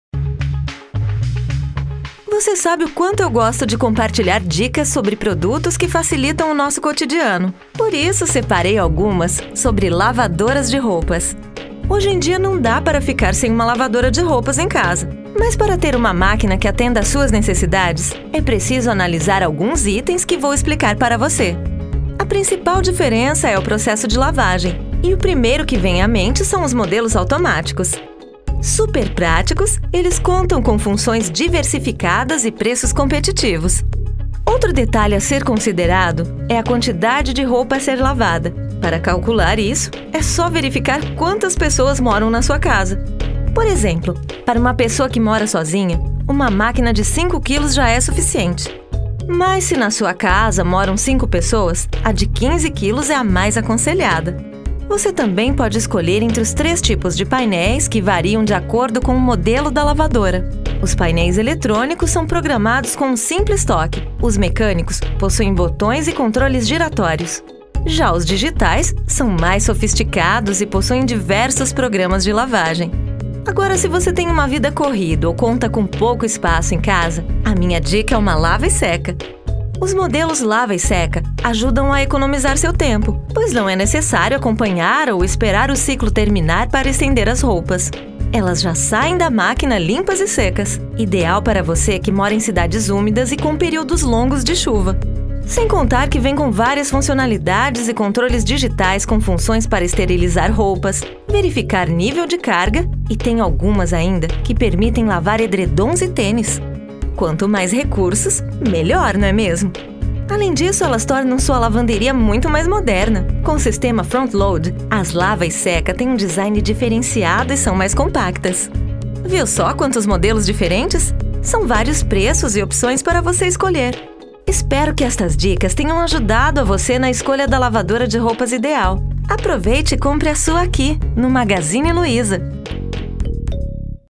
Saiba mais sobre Lavadoras e Secadoras
Confira as dicas sobre lavadoras e secadoras que a Lu separou para você. Aproveite e compre a sua aqui, no Magazine Luiza!